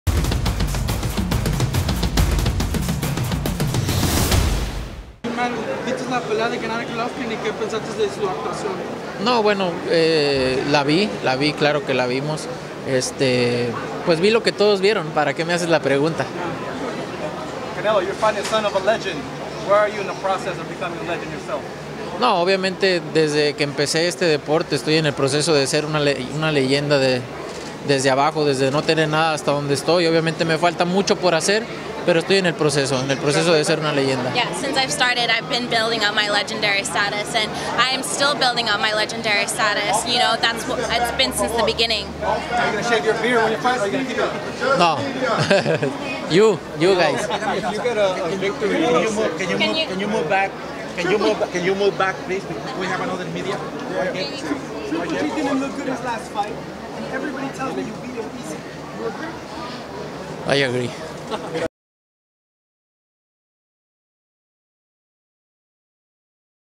Canelo Alvarez agrees that he can beat Gennady Golovkin easily!
In this Canelo vs. Golovkin video, Canelo Alvarez speaks on the Golovkin vs Jacobs fight and feels he beats Golovkin easily